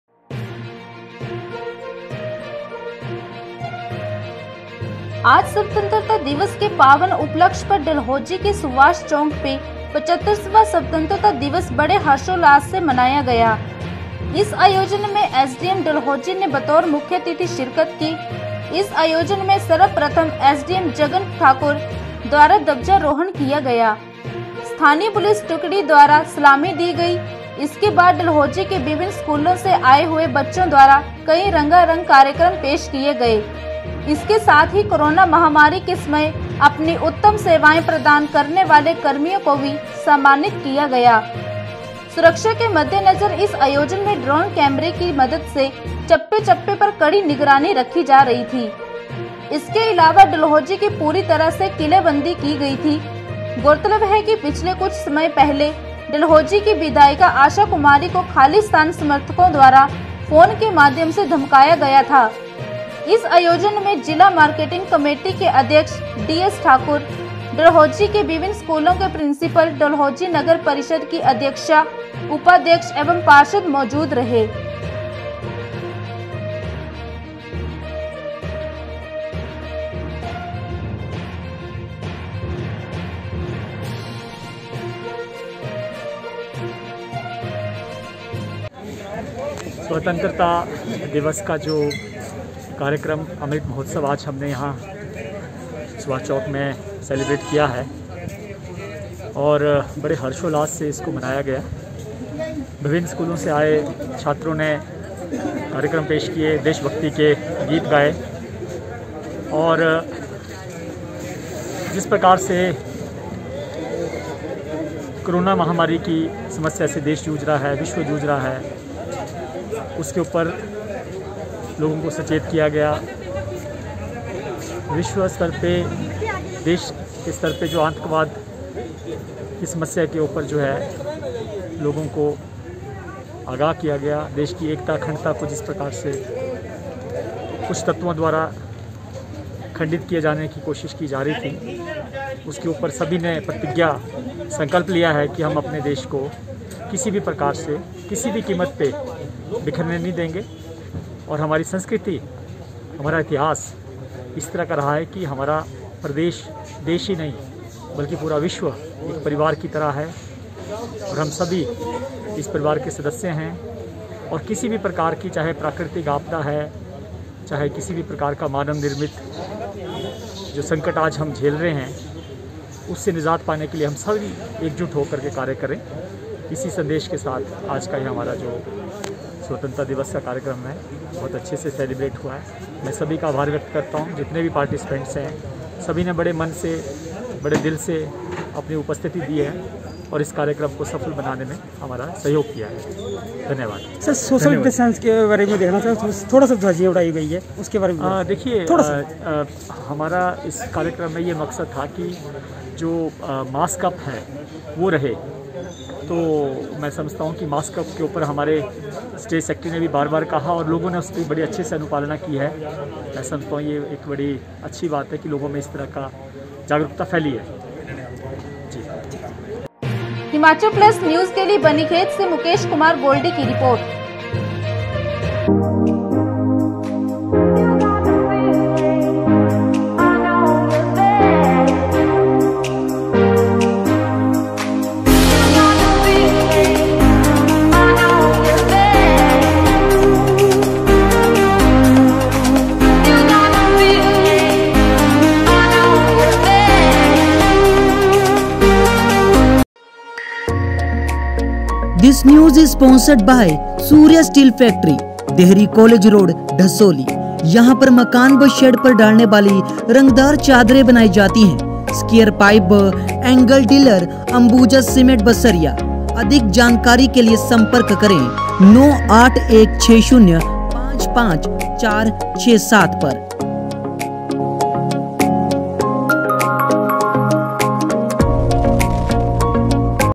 डलहौजी में धूमधाम से मनाया गया 75वां स्वतंत्रता दिवस
@Himachal Plus News